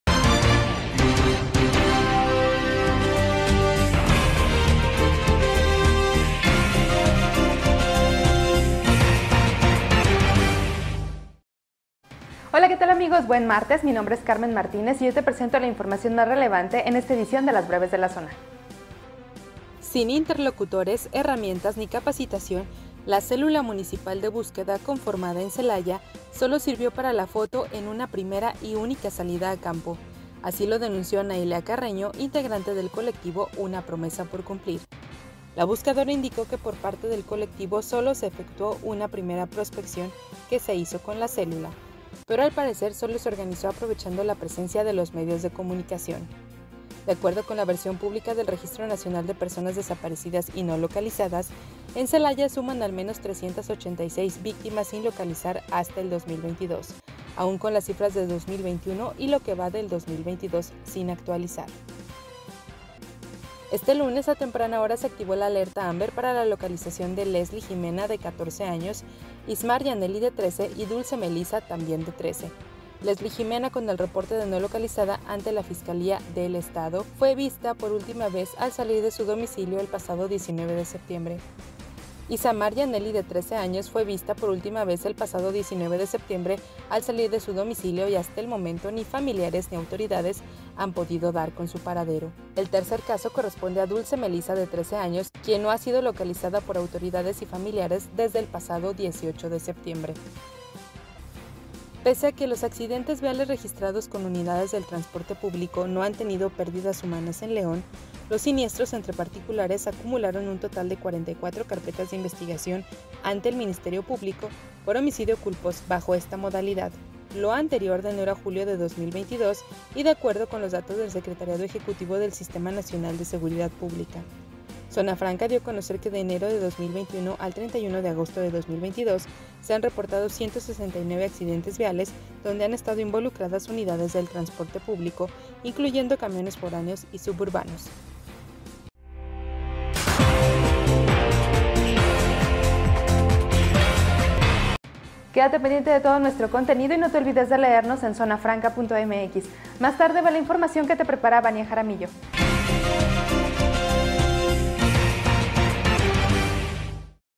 Accidentes viales en León deja al menos 44 muertos de enero a julio | BrevesDeLaZona📰‼️
Esto es LoÚltimo en Guanajuato
- Accidentes viales en León deja al menos 44 muertos de enero a julio
- Colectivo denuncia inactividad de la célula de búsqueda en Celaya: “Solo fue para la foto”